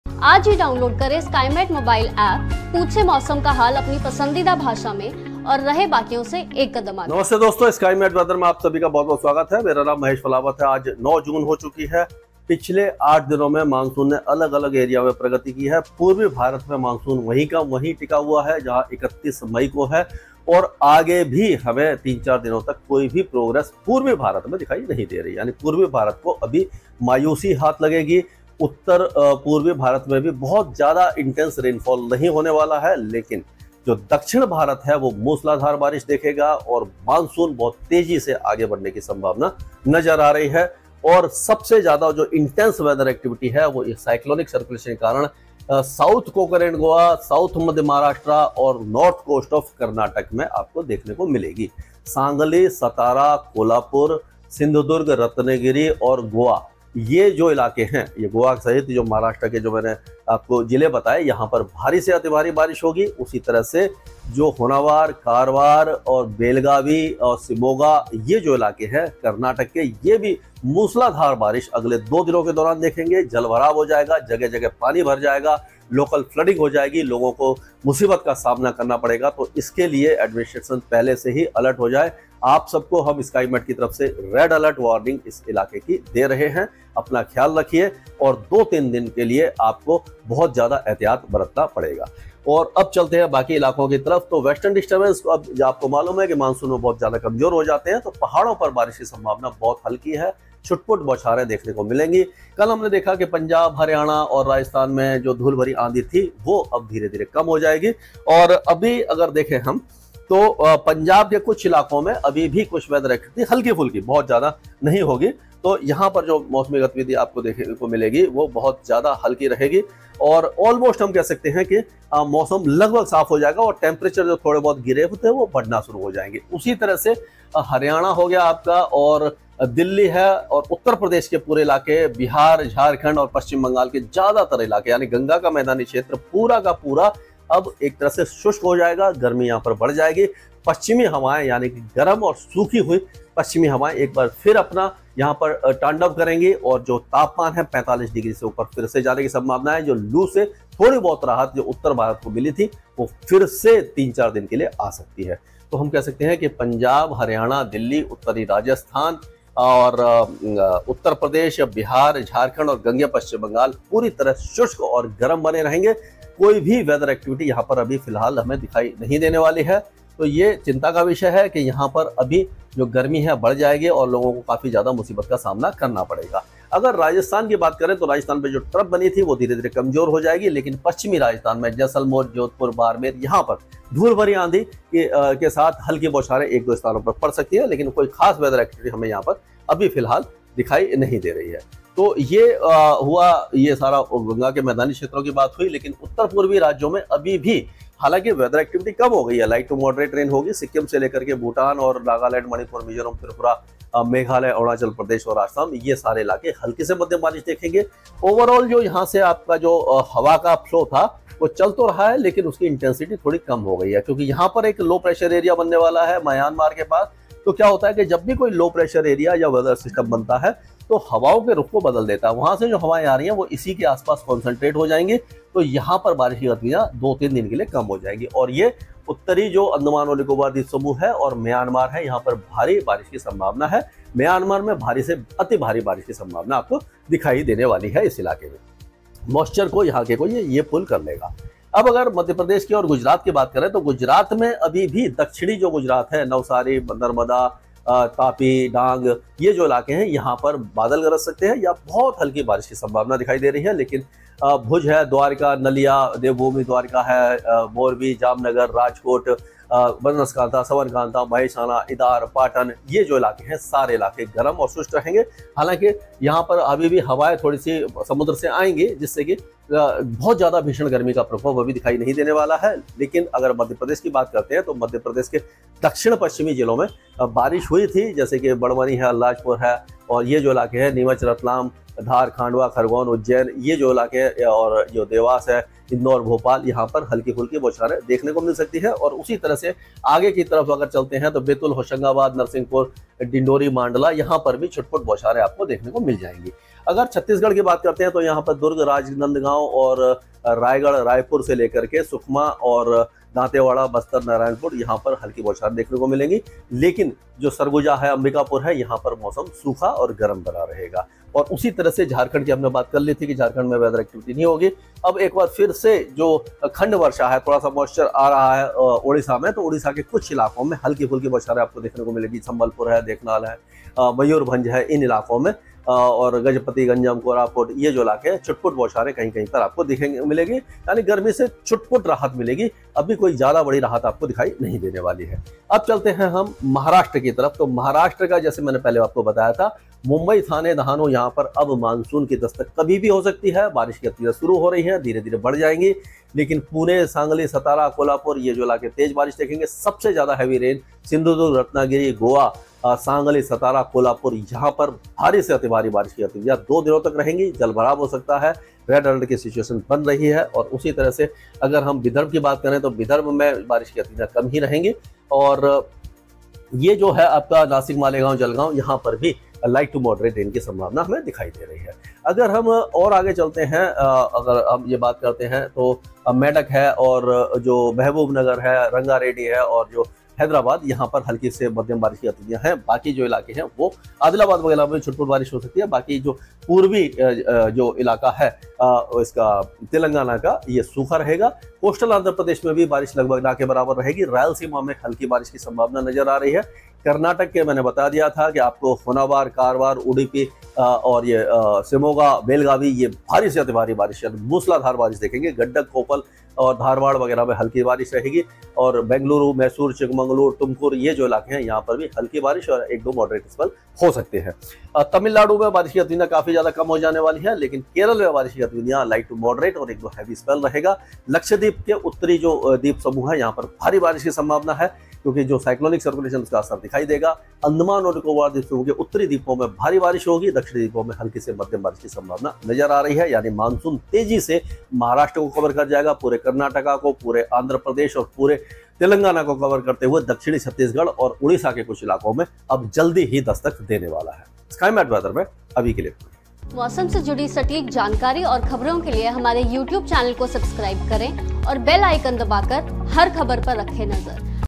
आज मुंबई में मानसून की दस्तक। कई राज्यों में भारी बारिश। उत्तर भारत में फिर हीटवेव | Skymet Weather
मुंबई में बारिश की गतिविधियां शुरू हो चुकी है अब आज 9 जून को हमारा अनुमान है कि मुंबई और पुणे तक मानसून पहुंच जाएगा। अगले दो दिनों के दौरान कर्नाटक के उत्तरी तट सहित दक्षिणी महाराष्ट्र में भारी से अधिक भारी बारिश हो सकती है। पूर्वोत्तर राज्यों में हल्के से मध्यम बारिश जारी रहेगी। दक्षिण भारत में भारी बारिश की गतिविधियां जारी रह सकती हैं। पिछले कुछ दिनों से अरब सागर से आने वाले नम हवाएं उत्तर और उत्तर पश्चिम भारत में धूल भरी आंधी और हल्की बारिश दे रही थी जिससे तापमान कुछ काम हुए थे। अब एक बार फिर हवाओं की दिशा पश्चिम हो जाएगी यह गर्म और शुष्क हवाएं फिर से उत्तर भारत में हीट वेव ला सकती हैं।